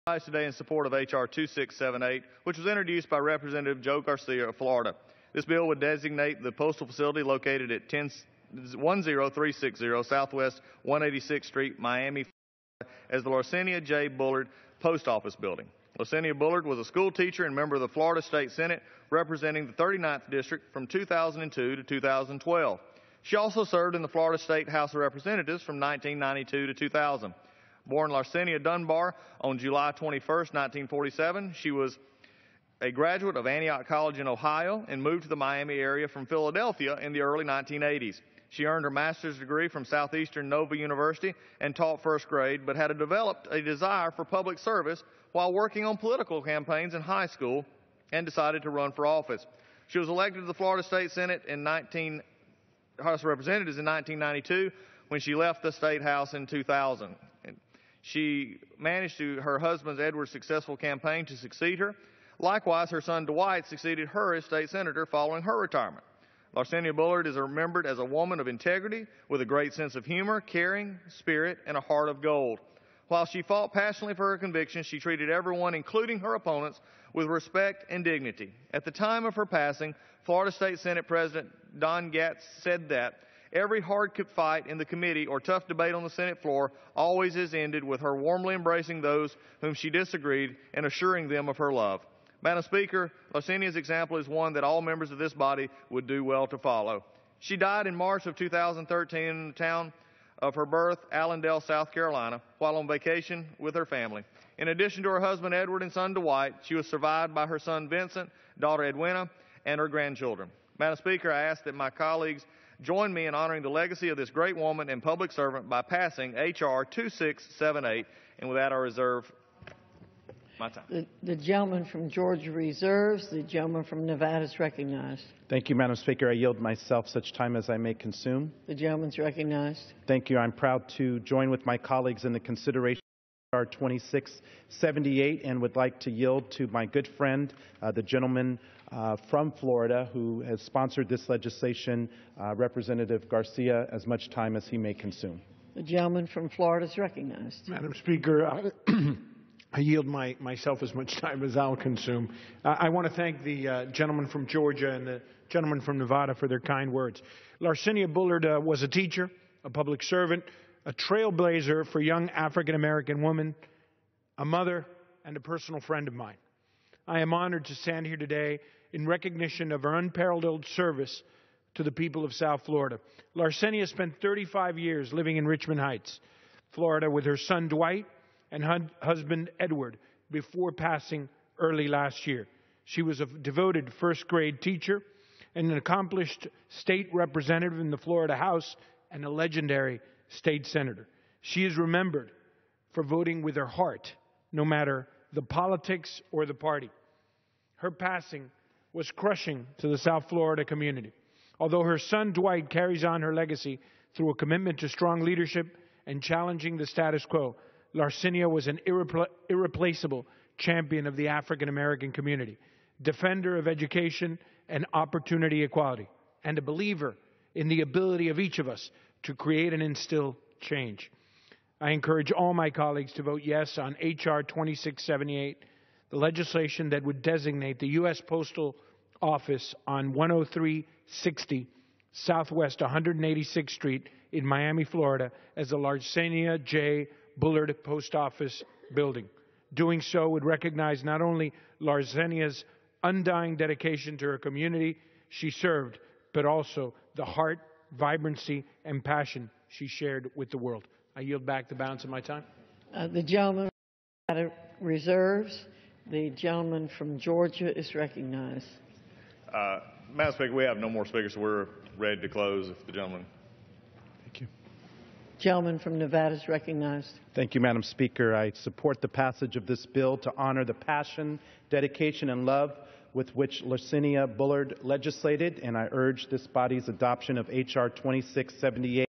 Congressmen Garcia, Collins, and Horsford Speak to support Larcenia Bullard Post Office
Congressmen Joe Garcia, Doug Collins (R-GA), and Steven Horsford (D-NV) speak in support of Congressman Garcia's legislation to name a post office in Miami after the late State Senator Larcenia J. Bullard.